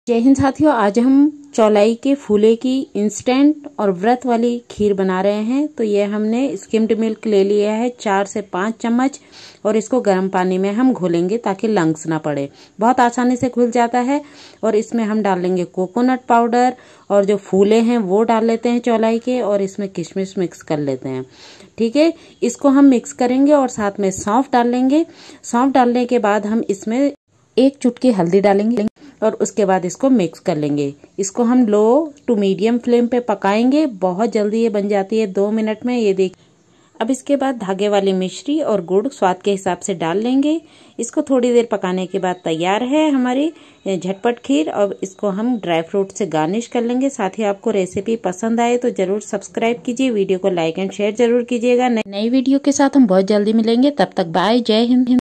बिन तैयारी, बिन झंझट के 2 मिनट में तैयार करें व्रत वाली खीर। Instant Kheer For Fasting.
किसी तैयारी के झंझट के बिना 2 मिनट में तैयार करें व्रत वाली खीर। Instant Kheer For Fasting.

चौलाई के फ़ूले या खील ऐसे बनाएं 👇